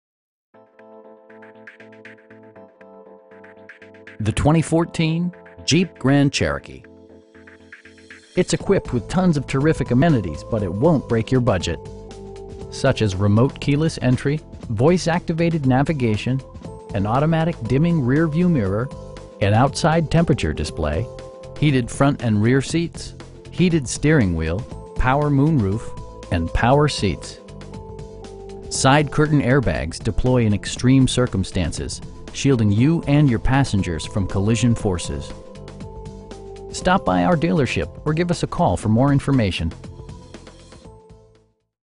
2014 Jeep Grand Cherokee Limited in Chicago, IL 60641
Marino Chrysler Jeep Dodge
5133 W Irving Park Rd

The 2014 Jeep Grand Cherokee. It's equipped with tons of terrific amenities, but it won't break your budget. Such as remote keyless entry, voice activated navigation, an automatic dimming rear-view mirror, an outside temperature display, heated front and rear seats, heated steering wheel, power moon roof, and power seats. Side curtain airbags deploy in extreme circumstances, shielding you and your passengers from collision forces. We pride ourselves on providing excellent customer service. Stop by our dealership or give us a call for more information.